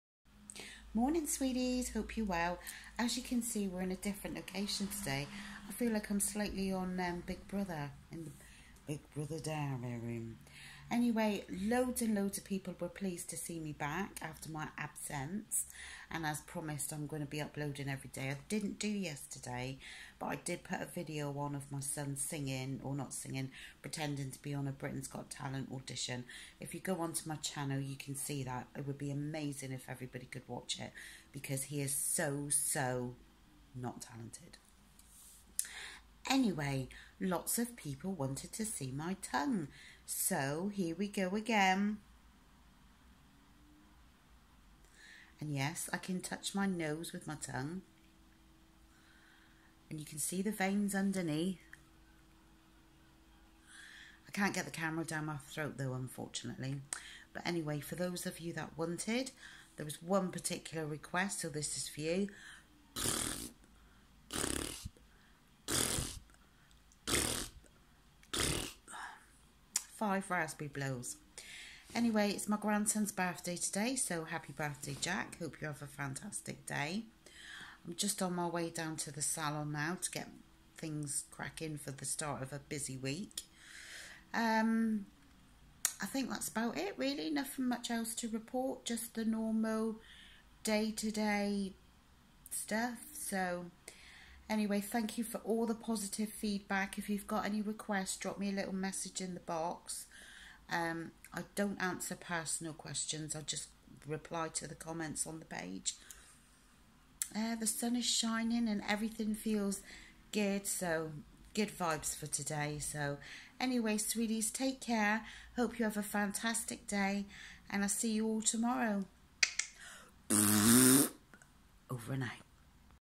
Wednesday vlog ....tongue twisters and raspberry blowing x
Kate's back again doing what she does best ...tongues and raspberry blowing x